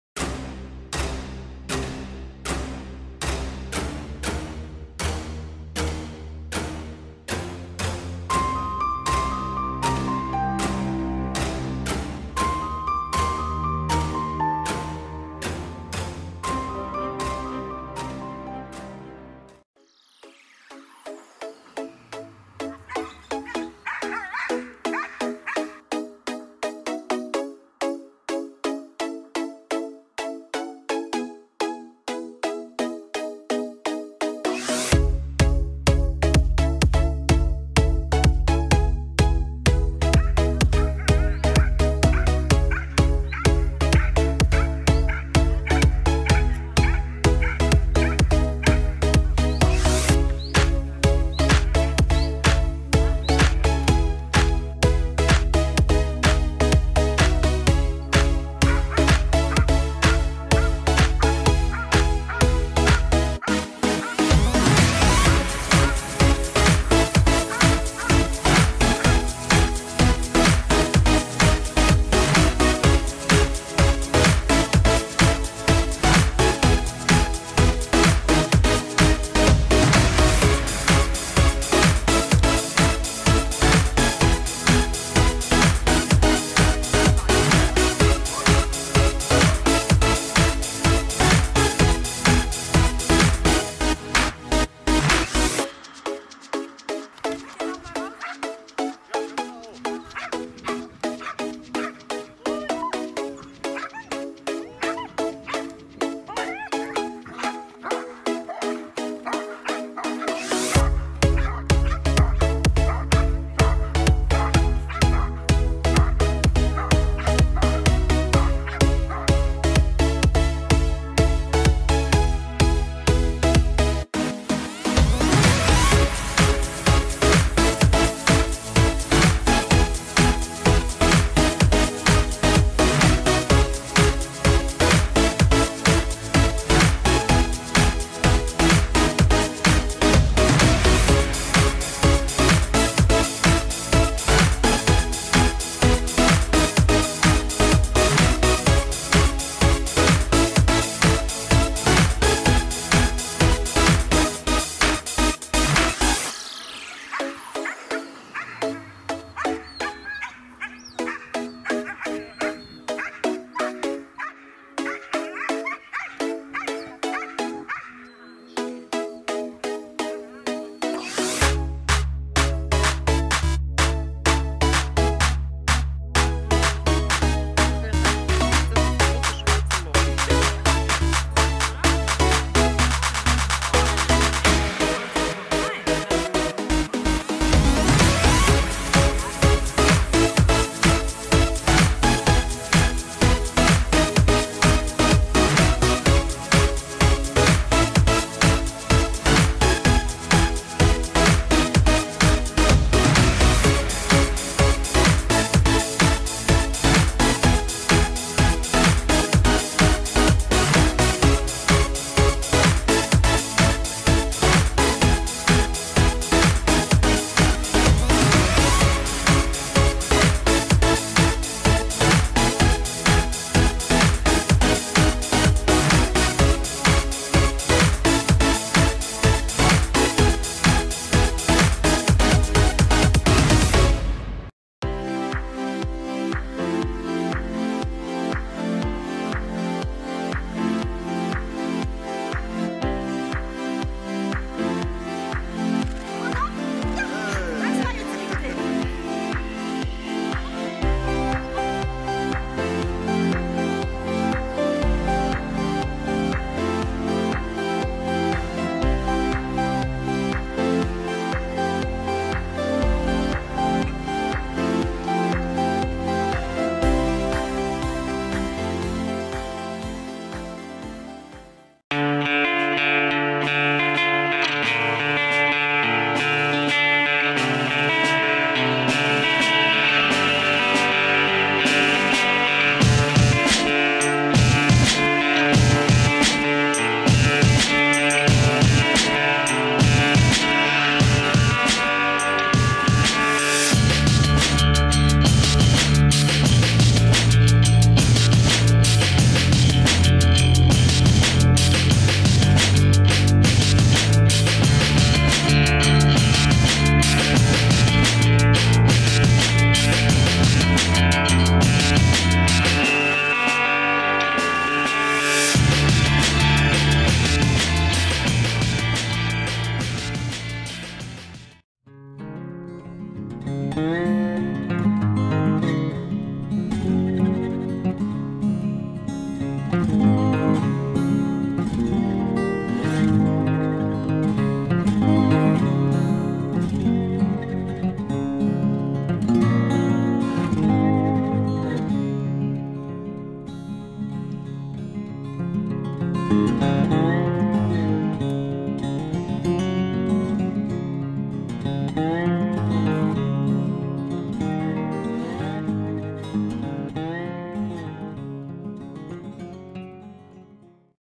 Malinois vom schnellen Fahnder - Schnelles Fahnder Treffen 2018
Ein Rückblick vom Fahndertreffen 2018 in Olsberg. Es gab jede Menge Action und es hat sich gezeigt, wie vielfältig die schnellen Fahnder eingesetzt werden: Im IPO, Agility, Mondioring, Obedience, in der Rettungshundearbeit oder einfach als toller Familienhund der Action liebt - es hat wieder großen Spaß gemacht. Bis nächstes Jahr!